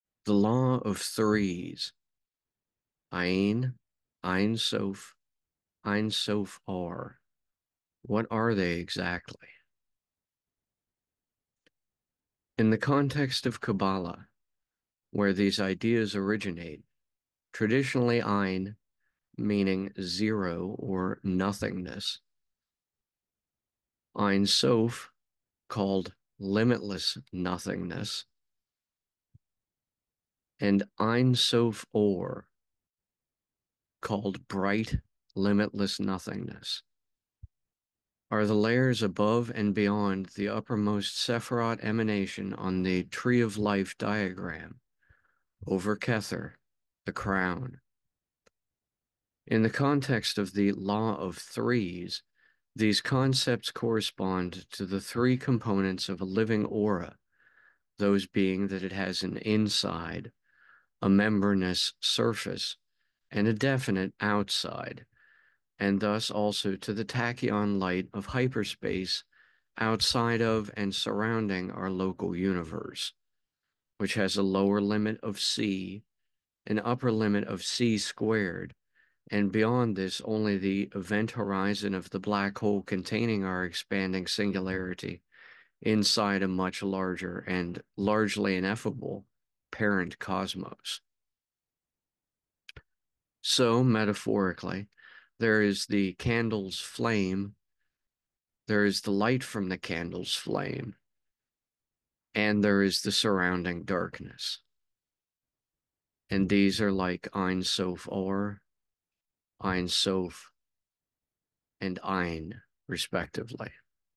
On Ayn, Ayn Soph & Ayn Soph Aur
PODcast 018 1

1. the law of threes: Ayin, ayin soph, ayin soph aur - what are they exactly?

In the context of QBLH, where these ideas originate, traditionally “Ayn” (meaning “zero” or “nothingness”), Ayn Soph (called “limitless nothingness”) and “Ayn Soph Aur” (called “bright limitless nothingness”) are the layers above and beyond the uppermost Sefirot emanation on the “tree of life” diagram - over “Kether,” the crown. In the context of the “law of threes,” these concepts correspond to the 3 components of a living “aura” (those being that it has an “inside,” a membranous “surface,” and a definite “outside”), and thus also to the tachyon light of hyperspace outside of and surrounding our local universe (which has a lower limit of “c,” an upper limit of “c^2,” and beyond this only the “event horizon” of the blackhole containing our expanding singularity inside a much larger, and largely ineffable, “parent cosmos”). So, metaphorically, there is the candle’s flame, there is the light from the candle’s flame, and there is the surrounding darkness - and these are like ayn soph aur, ayn soph, and ayn respectively.